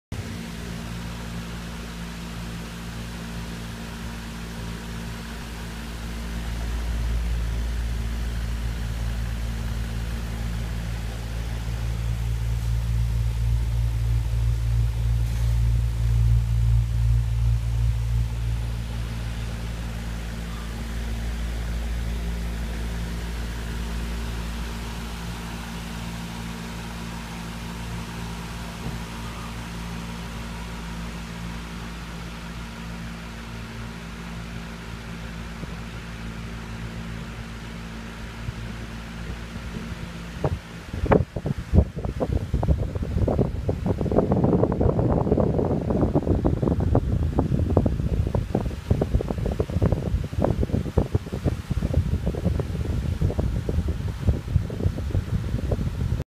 Rough Collie Shelbie in front of the fan
My dog Shelbie trying to beat the heat. Taped with my iPhone.